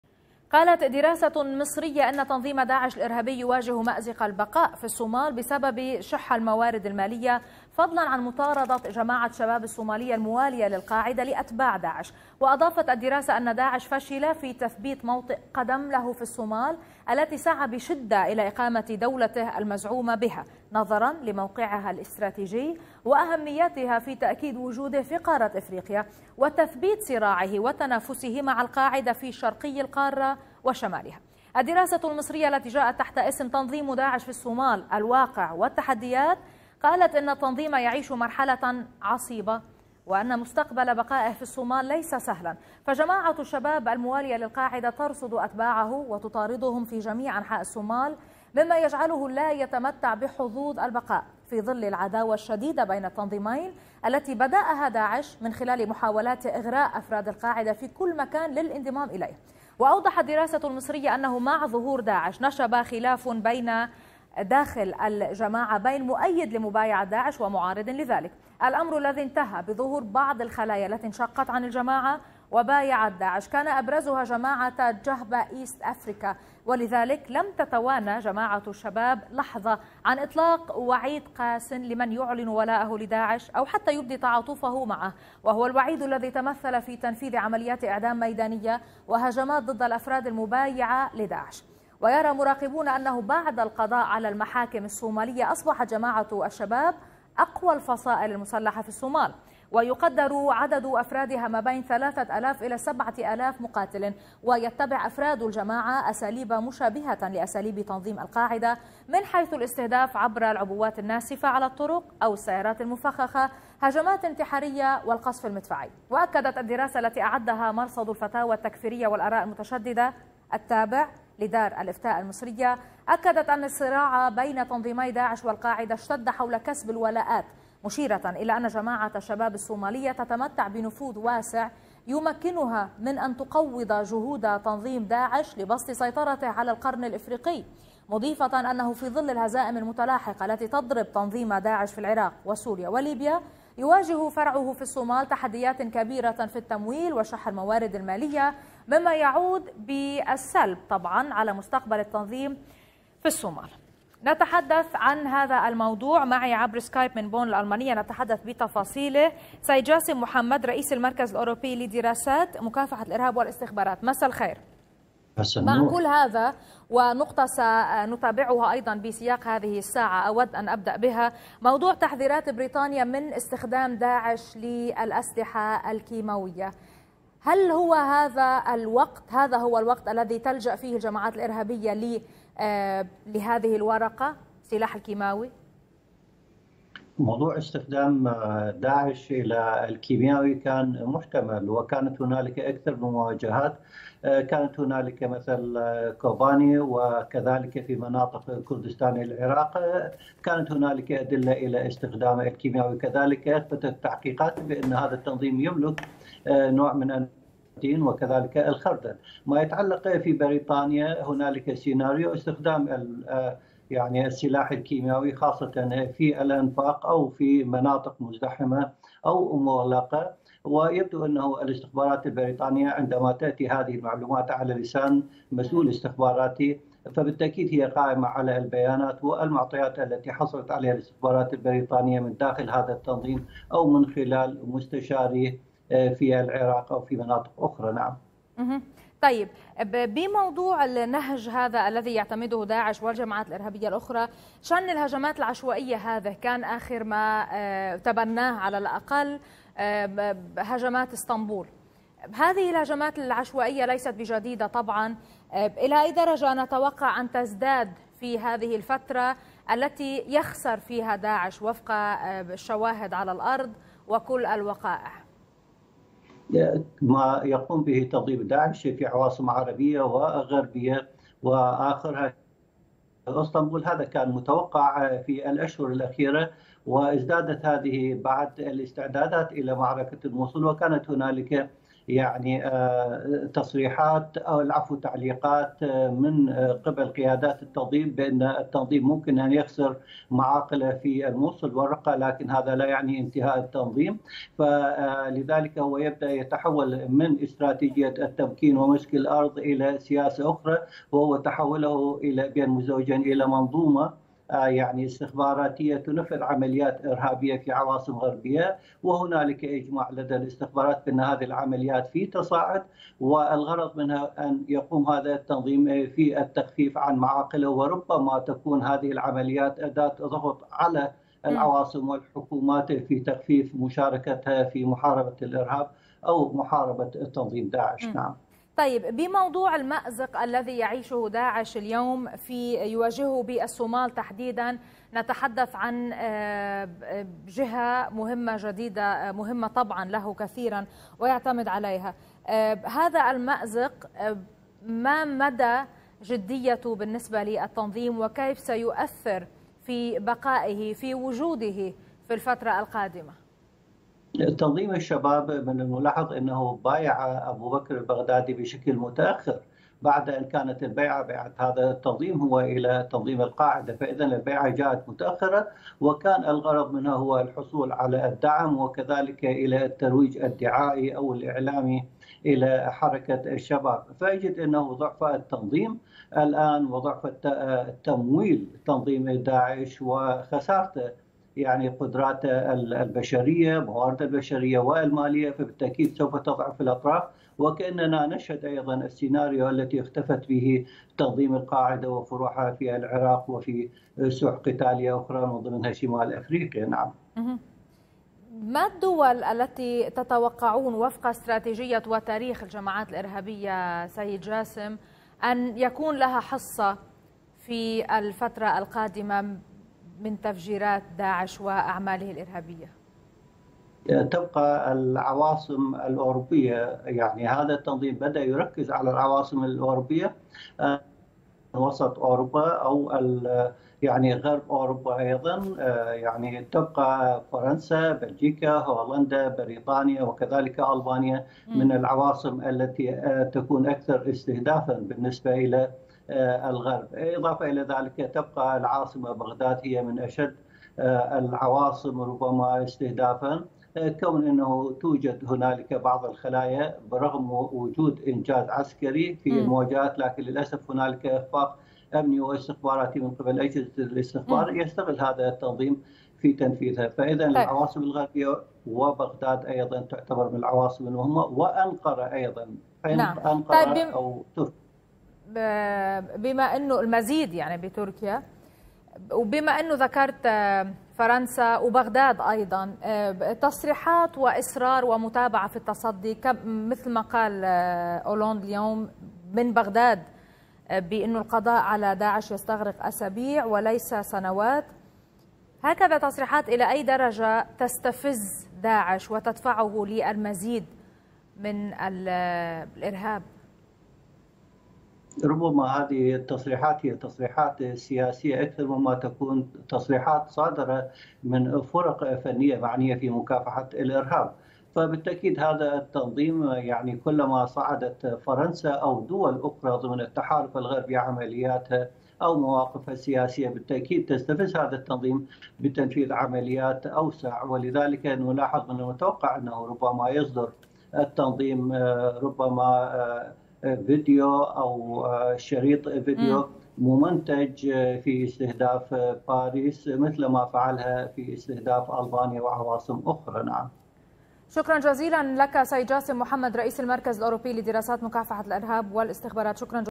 أخبار عربية | ما هي أسباب انكماش داعش في الصومال؟
شاهد آيضا:
. شاهد آيضا:
قيادي بعثي يفضح تاريخ آل الأسد!؟
قالت دراسة مصرية، إن تنظيم داعش الإرهابي يواجه مأزق البقاء في الصومال بسبب « ُشح» الموارد المالية، فضلا عن مطاردة جماعة «الشباب» الصومالية الموالية للقاعدة لأتباع داعش.

وأضافت الدراسة، أن داعش فشل في تثبيت موطئ قدم له في الصومال التي سعى بشدة إلى إقامة دولته المزعومة بها؛ نظرا لموقعها الاستراتيجي وأهميتها في تأكيد وجوده في قارة أفريقيا، وتثبيت صراعه وتنافسه مع القاعدة في شرقي القارة وشمالها.

الدراسة المصرية، التي جاءت تحت اسم «تنظيم داعش في الصومال.. الواقع والتحديات»،قالت إن التنظيم يعيش مرحلة عصيبة.

وأن مستقبل بقائه في الصومال ليس سهلا، فجماعة الشباب الموالية للقاعدة، ترصد أتباعه وتطاردهم في جميع أنحاء الصومال؛ مما يجعله لا يتمتع بحظوظ البقاء، في ظل العداوة الشديدة بين التنظيمين، والتي بدأها داعش من خلال محاولاته إغراء أفراد القاعدة في كل مكان للانضمام إليه.

ليبيا داعش سوريا العراق